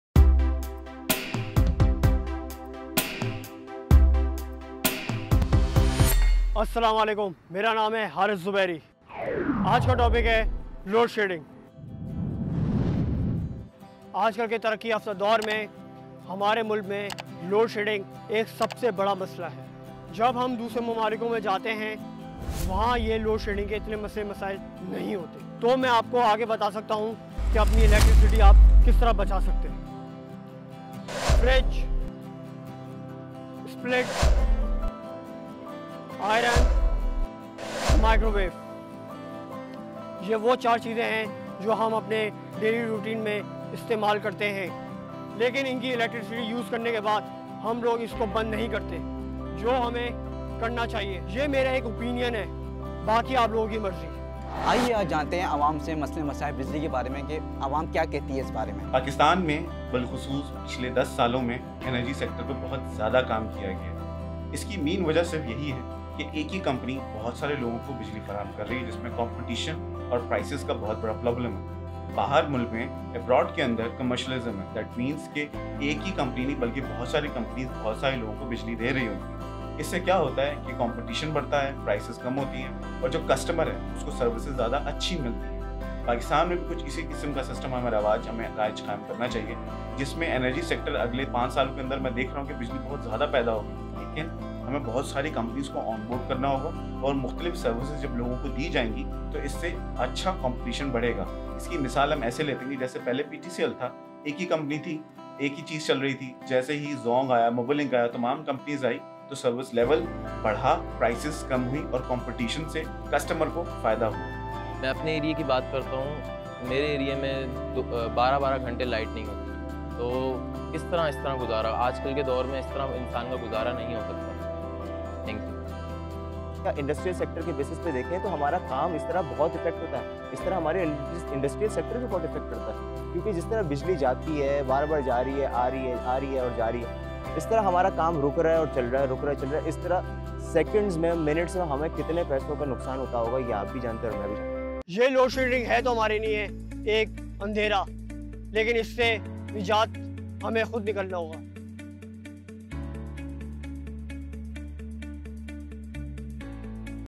How To Save On Your Electricity Bill
Load shedding (load shedding) is a way to distribute demand for electrical power across multiple power sources. Load shedding is used to relieve stress on a primary energy source when demand for electricity is greater than the primary power source can supply. Most buildings, including data centers, purchase electrical power from a utility provider. To reduce the cost of power, while also ensuring continuous operation, a building operator may negotiate an agreement with the power provider to voluntarily load the shed on a pre-scheduled or on-demand basis. During load shedding events, the building draws power from its secondary sources rather than from the utility. A typical secondary source is on-site diesel generators, or on-site or contracted solar photovoltaics, or wind-based renewable power. Power customers may experience involuntary load shedding when a utility electrical provider lowers or stops electricity distribution across the coverage area for a short period of time; this type of load shedding is commonly referred to as a rolling blackout. Brownouts, another type of involuntary load shedding, are caused by the power supplier lowering voltage distribution during peak usage times to balance supply and demand.